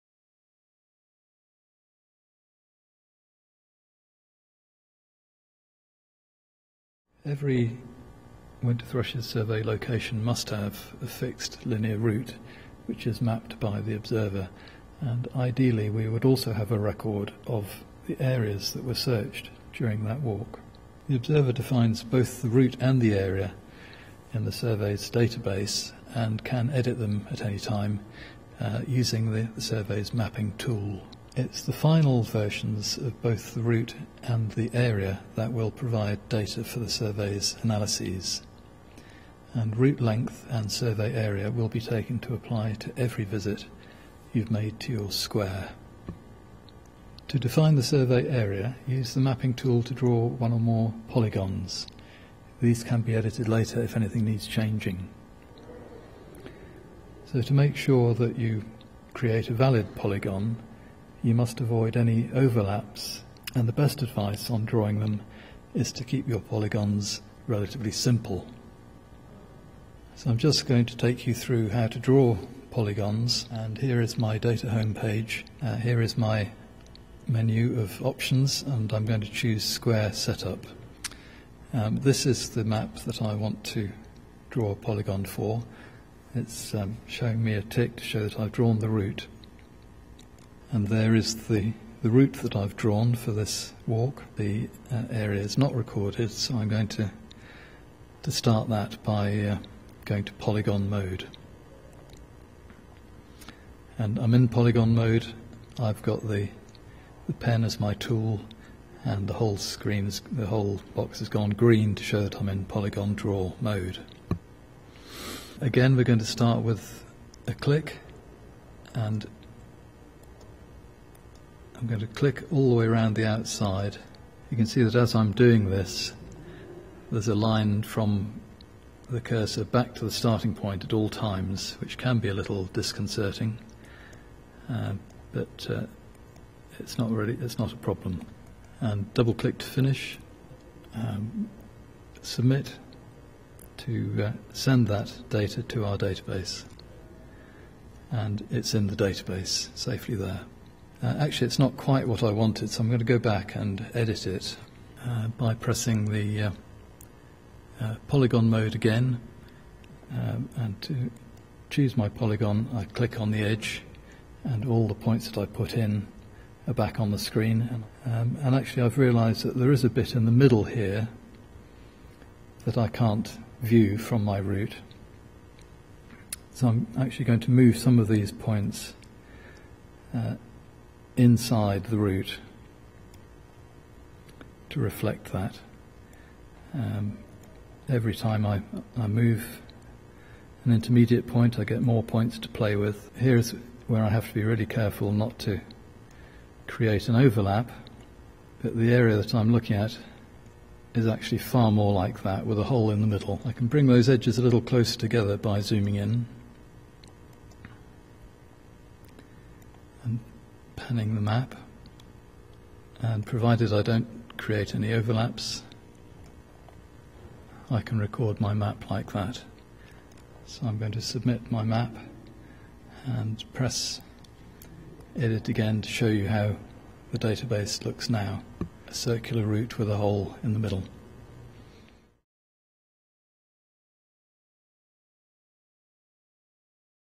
Winter Thrushes Survey - 3. Mapping and editing a survey area
Part 3 in our series of video tutorials to help get involved in the BTO Winter Thrushes Survey. Following from plotting your 'Winter Walk' route, this video shows how to define the visible survey area viewable along your route.